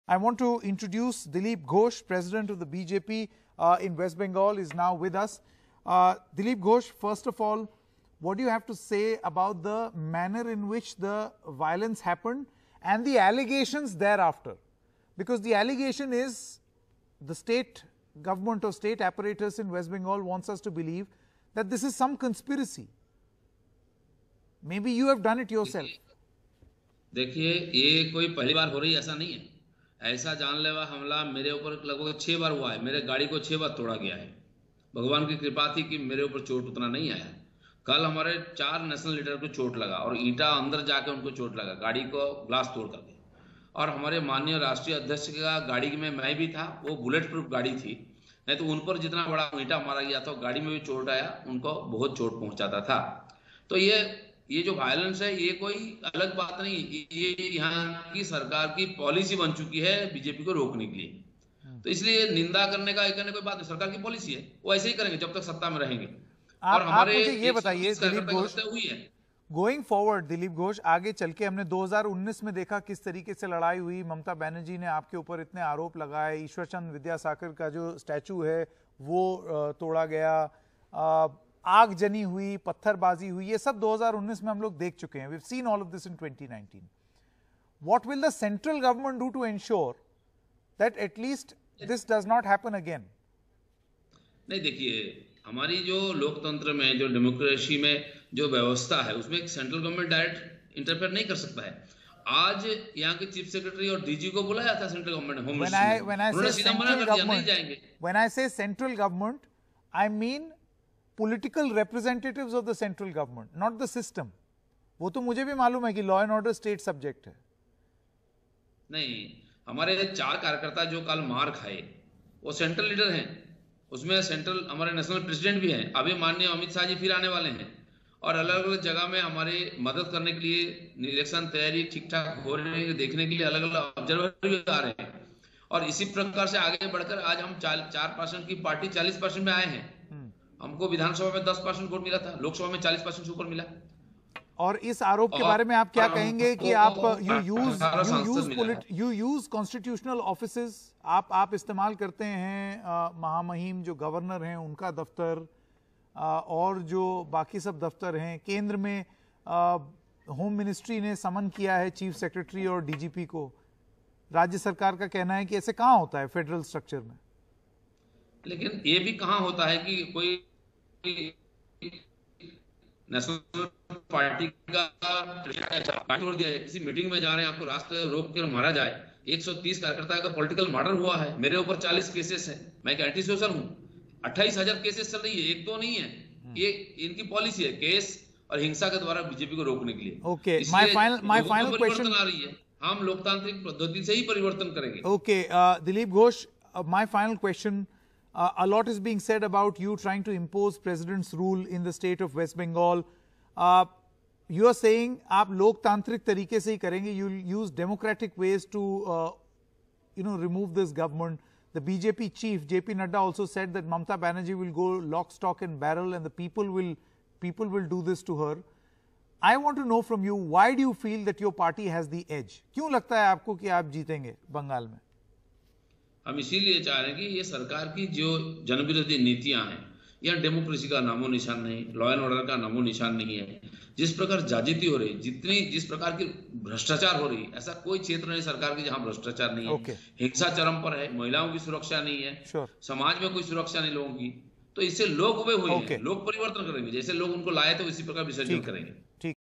Violence Has Become West Bengal Government's Policy Against BJP: Dilip Ghosh
Referring to an attack on BJP chief JP Nadda's convoy on Thursday, the party's Bengal chief, Dilip Ghosh, in an interview with NDTV, said that violence has become the Mamata Banerjee government's policy against the BJP.